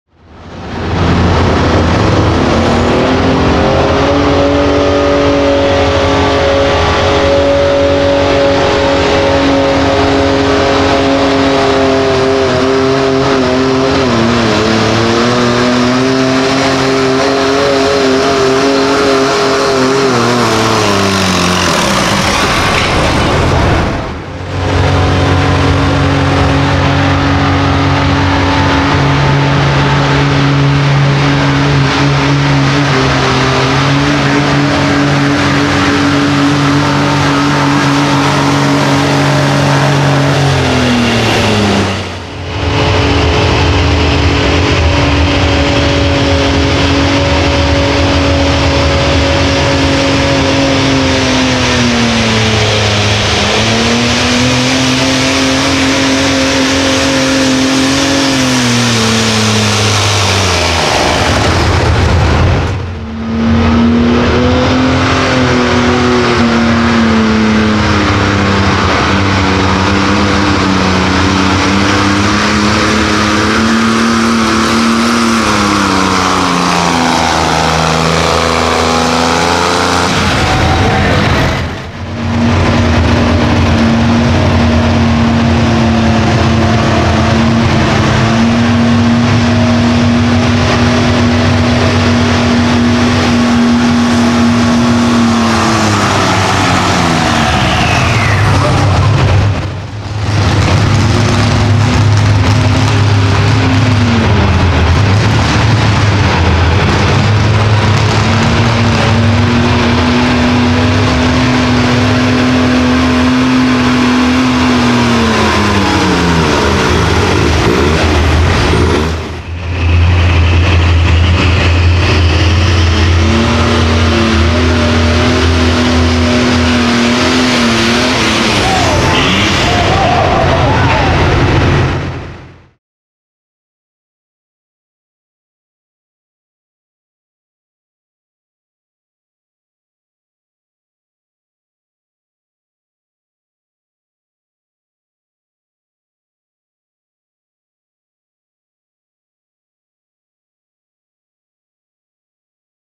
1100 Pro Stock Diesel Tractors Pulling at the Midwest Spring Super Pull - Tractor Pulling 2023
Midwest Spring Super Pull
Cedar Lake Indoor Arena

1100 Pro Stock Diesel Tractors pulling at the 2023 Midwest Spring Super Pull in New Richmond, WI